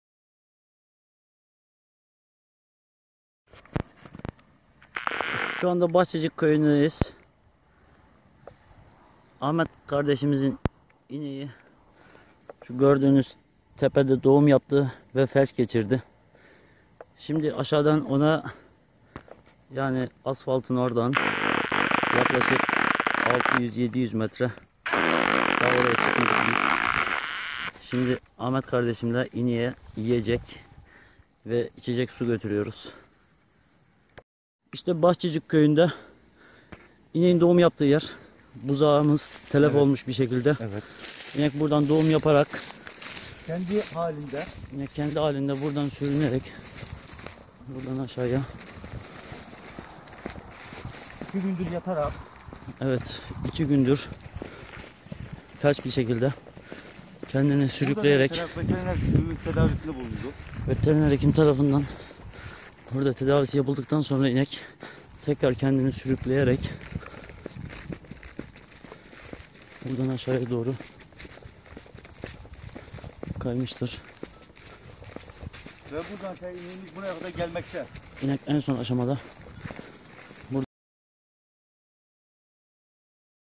Ormanda doğum yaptı, felç geçirince mahsur kaldı
Bartın'ın Ulus ilçesine bağlı Kalecik Köyünde ormanlık alanda doğum yapan ve buzağısı ölü doğan inek felç geçirince mahsur kaldı.